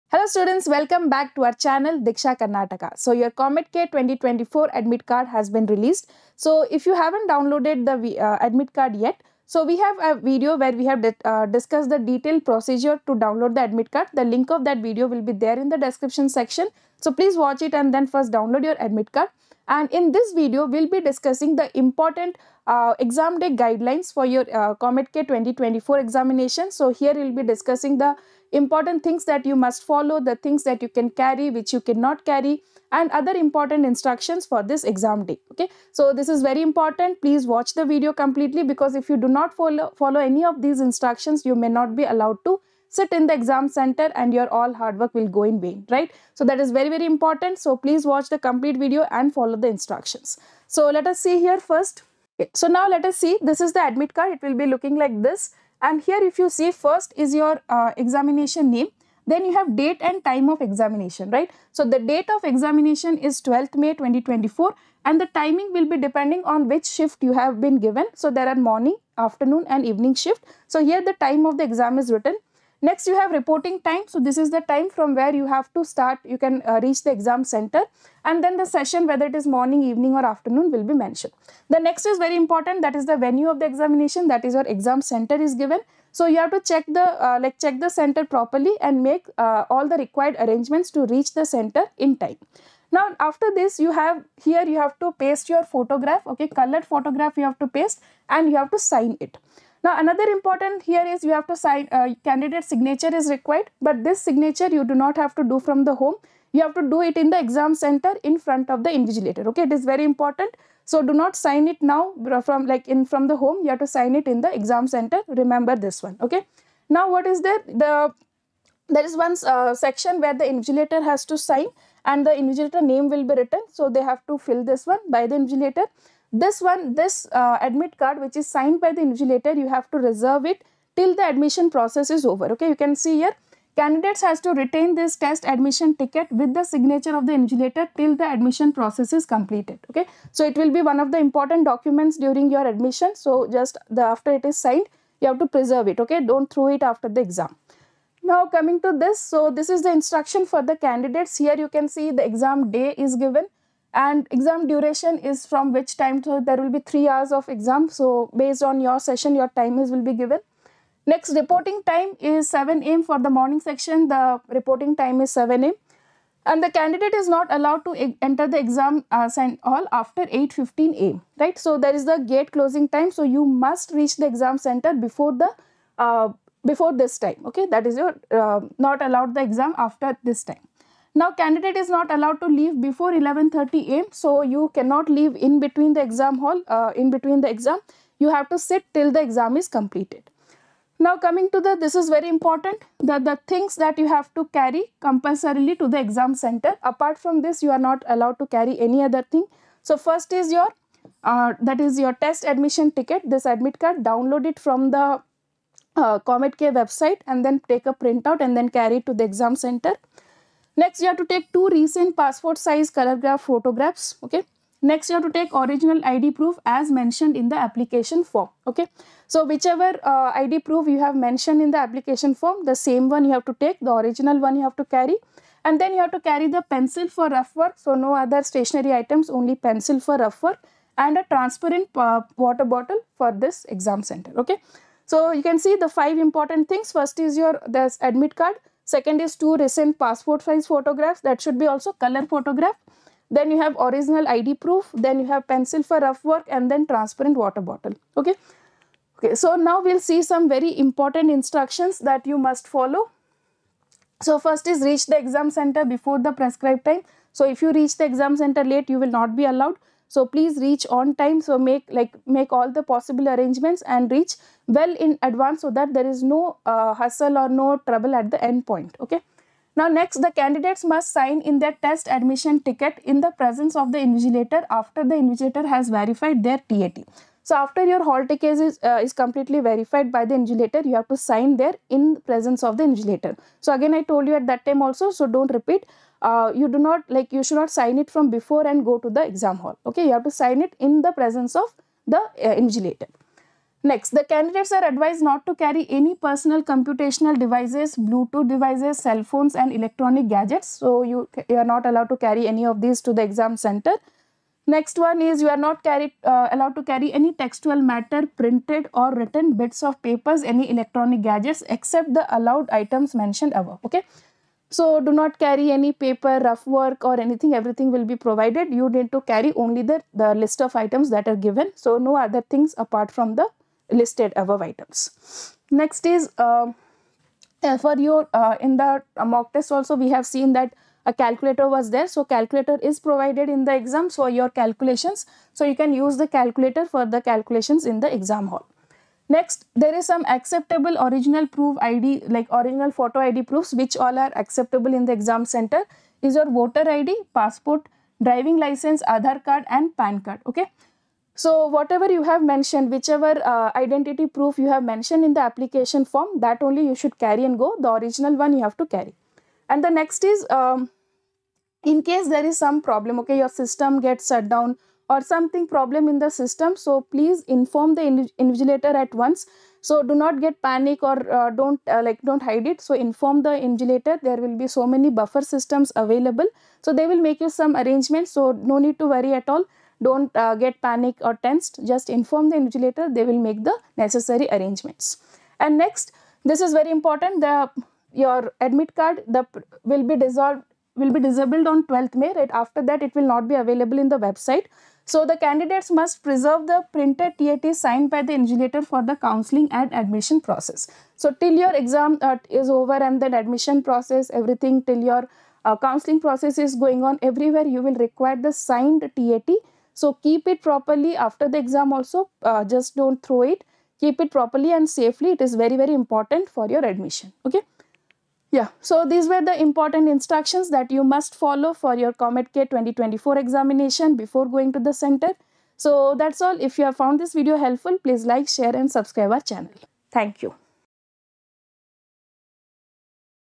COMEDK 2024 Exam Day Guidelines: What to Carry & Important Instructions
Hey COMEDK 2024 aspirants! The COMEDK 2024 admit card is out, and it's crucial to adhere to the instructions outlined in it for exam day. In this video, Aishwarya Ma'am discusses all the essential guidelines and instructions for the COMEDK 2024 exam day. Learn about exam timings, permissible items to carry, and what to avoid bringing to the exam hall. If you're gearing up for the COMEDK 2024 exam, these are vital details you must know before stepping into the exam hall.
Timestamps
00:00 COMEDK Exam Day Guidelines
00:55 COMEDK Admit Card
04:09 COMEDK 2024 Important Instructions for Exam Day